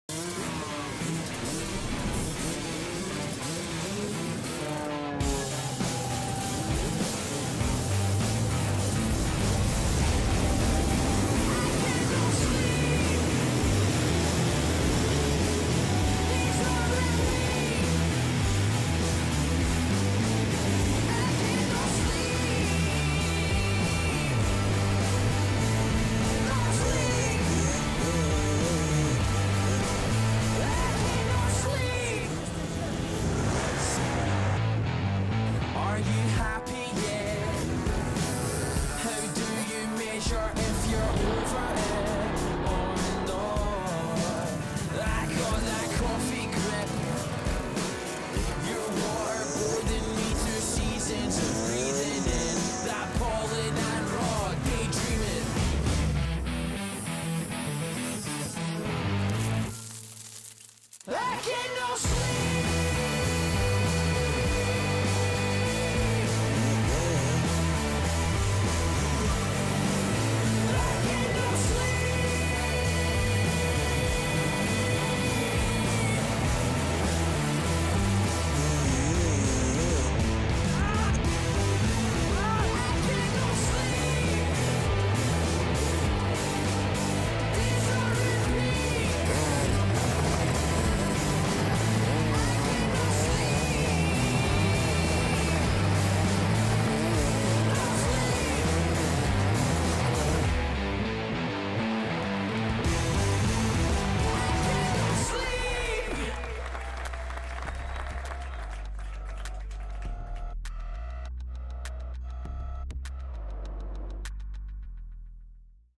Red Bull 111 Megawatt - oficjalny klip
Pot, krew i łzy - tak można skwitować tegoroczna edycję Red Bull 111 Megawatt. Spośród 500 zawodników zakwalifikowanych do niedzielnego finału tylko 18 udało się dojechać na metę! Po zaciętej walce, kiedy to cały czas zmieniali się liderzy pościgu, zwyciężył Jonny Walker. Tym samym, Brytyjczyk obronił swój ubiegłoroczny tytuł. Najlepszy spośród Polaków był Tadeusz Błażusiak, zajmujący 5. lokatę.